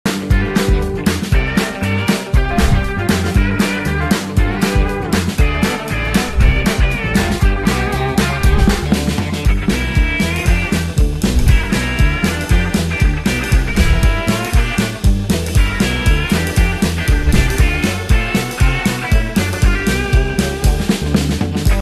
Coffee making | Coffee varieties | Loaves & Muffins | Trissur
Wafting through the air, the aroma of freshly brewed coffee welcomes you. Enjoy your cup  with a savoury bite  or pastry.